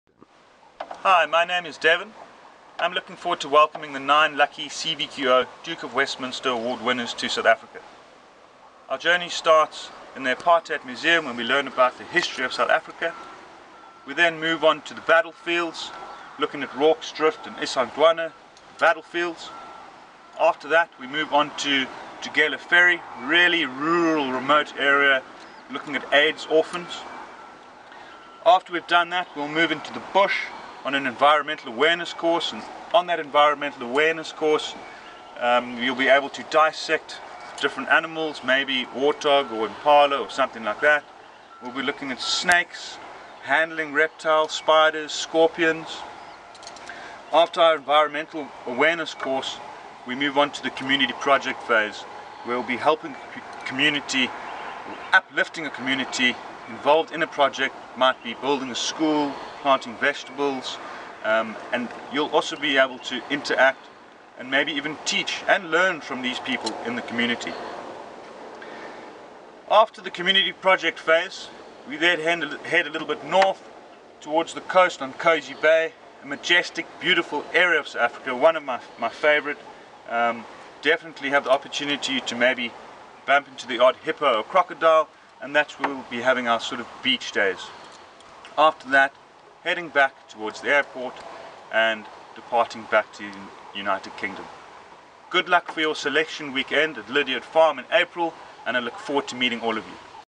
Duke of Westminster 2015 - Devin Abrahamson talks about the South Africa trip
With the Duke of Westminster selection event in Somerset just weeks away, the South Africa exped leader, Devin Abrahamson, has sent us this short film on what the nine lucky winners can expect during their two-week adventure.

Hopefully this will encourage the 24 regional finalists even more to do their very best at Outposts. Good luck, everyone.